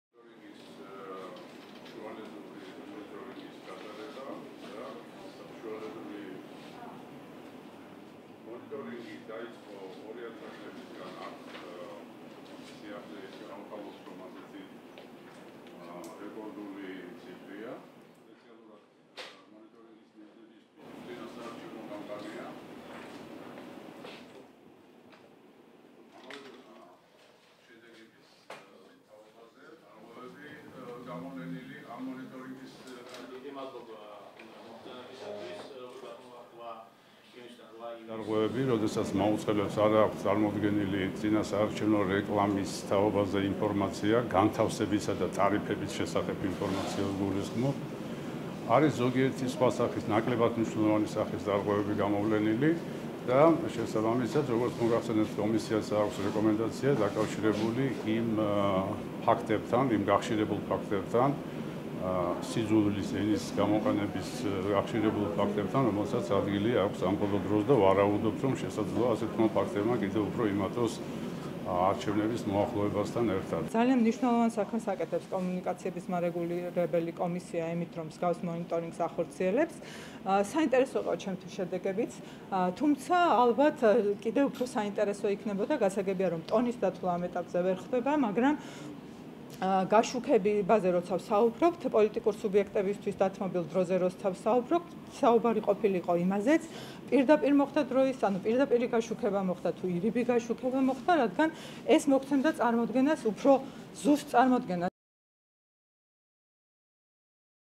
მონიტორინგის შედეგები /2030 (09.08.2016.)/
✔საქართველოს კომუნიკაციების ეროვნულმა კომისიამ წინასაარჩევნო პერიოდის მედიამონიტორინგის პირველი შუალედური ანგარიში წარადგინა.   53 ტელეკომპანიის და ცენტრალური და ადგილობრივი ბიუჯეტიდან დაფინანსებული გაზეთების მონიტორინგის შედეგად, სხვადასხვა შინაარსის დარღვევები გამოიკვეთა. მონიტორები  წინასაარჩევნო რეკლამას, აგიტაციას, დებატებს, ახალ და საზოგადოებრივ-პოლიტიკურ გადაცემებს  აკვირდებოდნენ.  ანგარიში 8 ივნისიდან 8 ივლისამდე პერიოდს მოიცავს,  მონიტორინგი 24- საათიან რეჟიმში არჩევნებამდე გაგრძელდება.